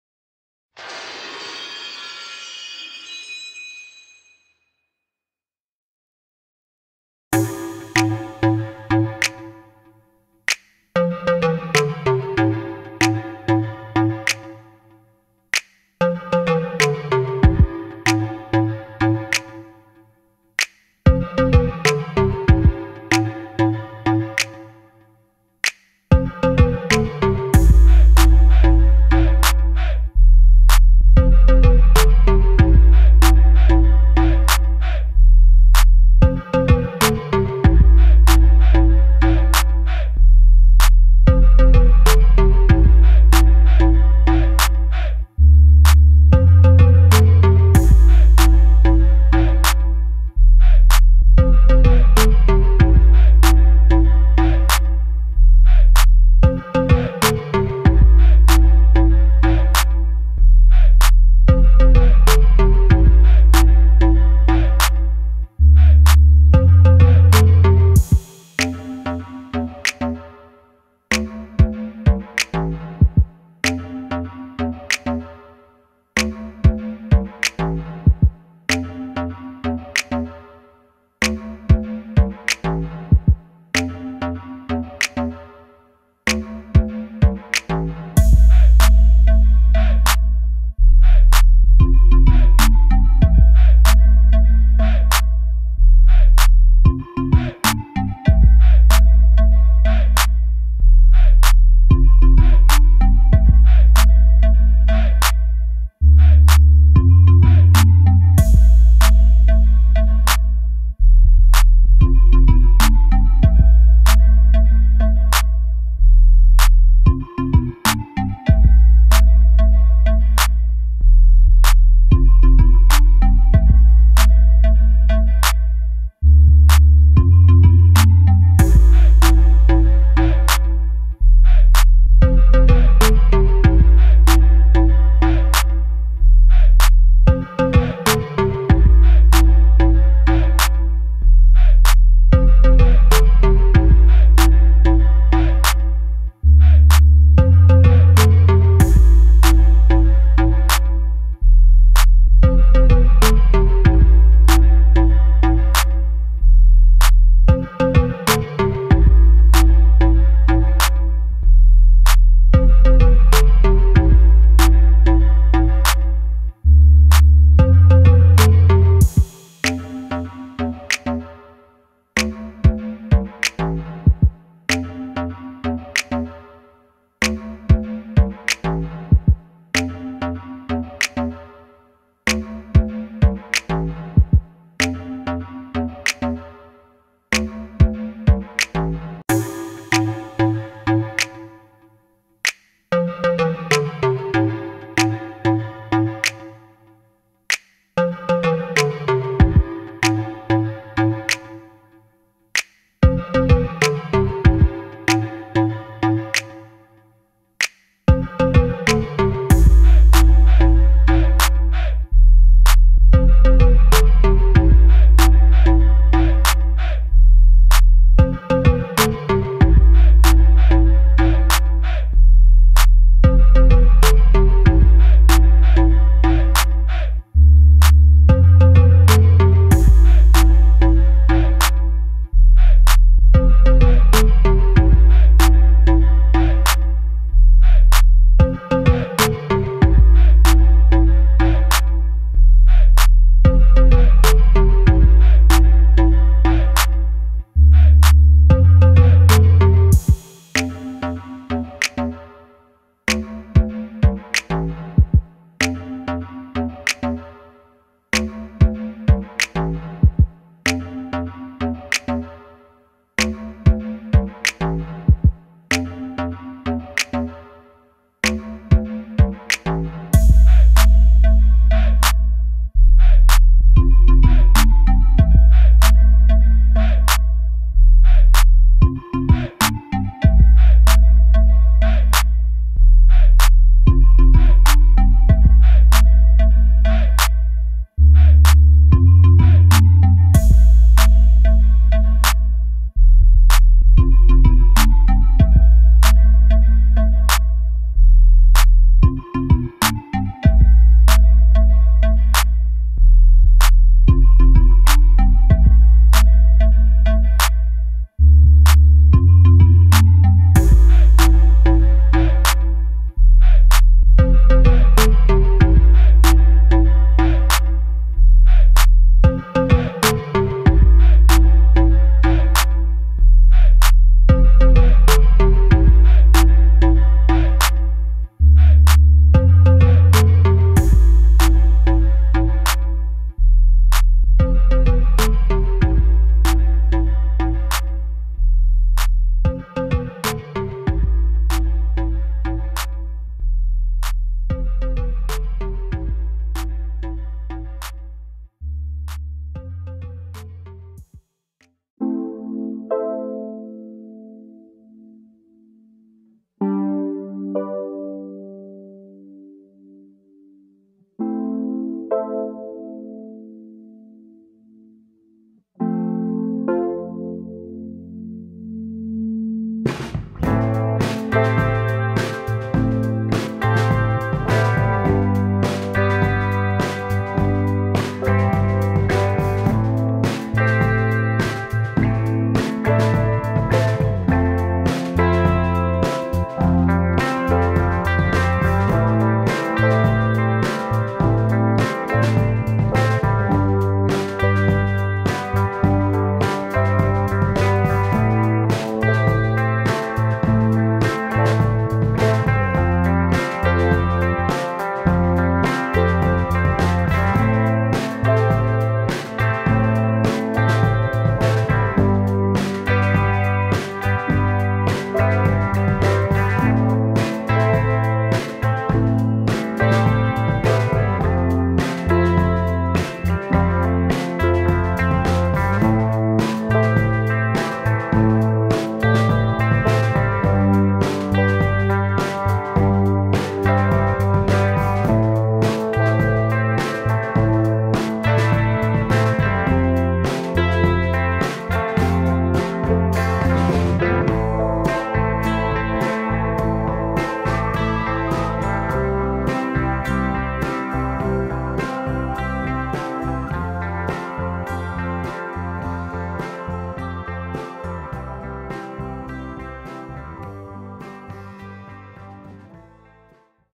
The best gift for a girl is a gold chain made with your own hands!
Making a 14-carat gold chain. 585. 6gr.  This is a very rare chain that you will not find on sale in any store. This exclusive chain is a good gift. You can buy this chain from me or order a similar one. This necklace will be a pride and unique decoration!